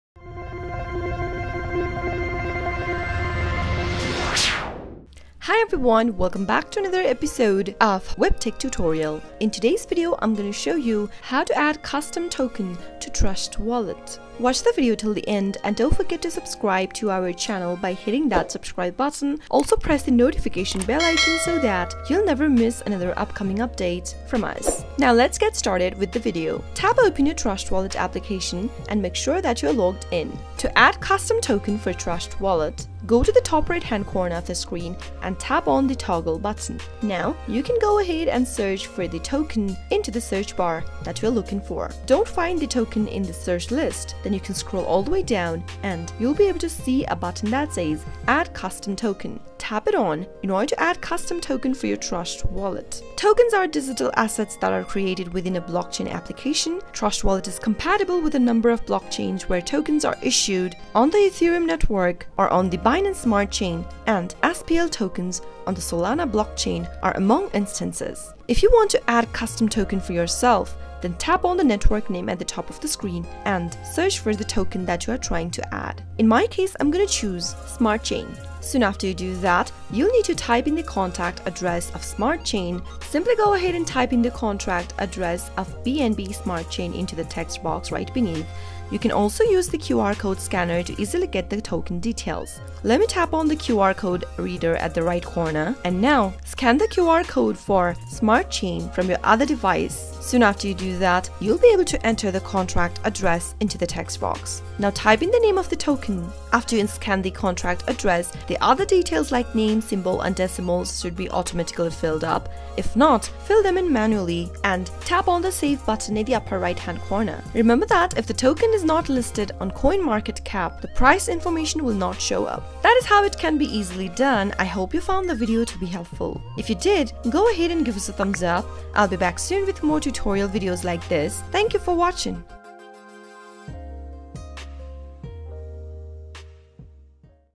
How To Add Custom Token to Trust Wallet (2022) | Add Custom Coins In Trust Wallet
KEEP YOUR CRYPTO CURRENCY SAFE Easy & updated methods on how to add custom token to Trust Wallet easily in 2 minutes with just internet connection.

CHAPTERS

0:24 - Add Custom Token To Trust Wallet
2:04 - Outro